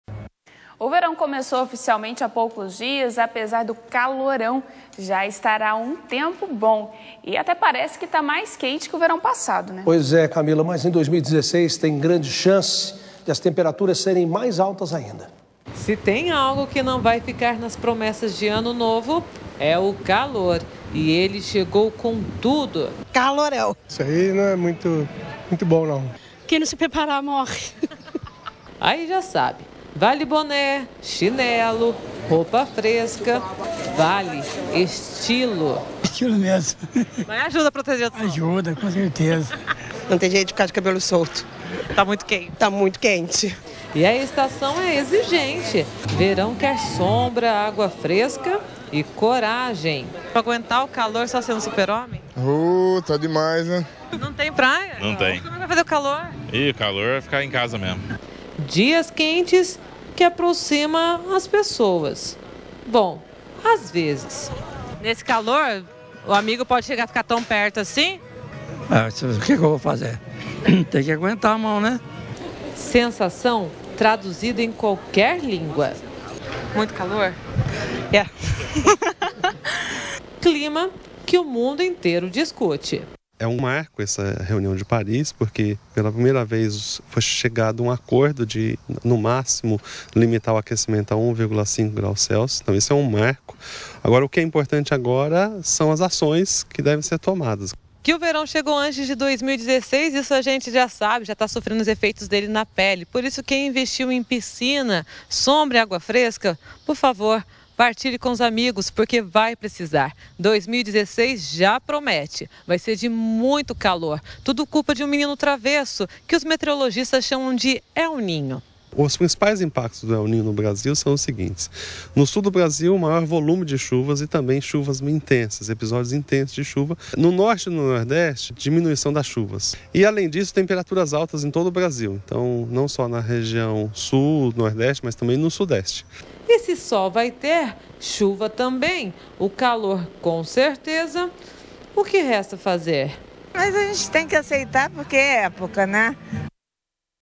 2016 deve ter temperaturas mais altas, comparadas às de 2015 - CN Notícias
O verão começou oficialmente no dia 22 de dezembro, apesar do calorão já estar há muito tempo. E até parece que tá mais quente que o verão passado. Mas 2016 tem grande chance de as temperaturas serem ainda mais altas.

Reportagem de Flávia Pereira e Genilson Pacetti